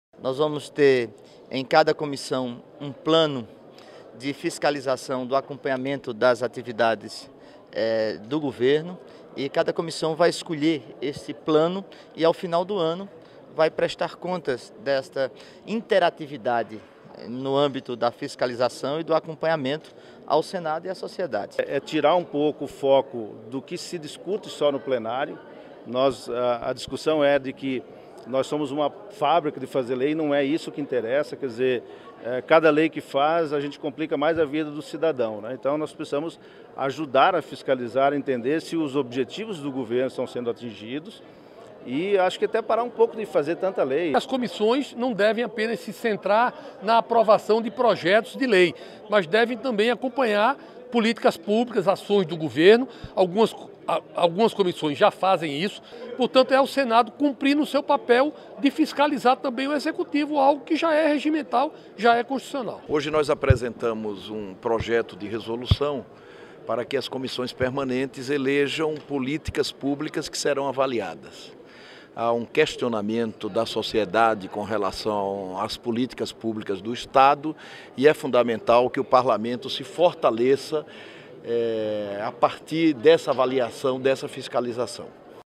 Comissões irão definir políticas públicas que serão fiscalizadas
O presidente do Senado, Renan Calheiros, apresentou nesta quarta-feira aos presidentes das comissões permanentes da Casa um projeto de resolução que estabelece a avaliação e fiscalização sistemática das políticas públicas do governo. Cada comissão irá definir o plano de acompanhamento das atividades do governo e ao final do ano prestará contas ao Senado e à sociedade.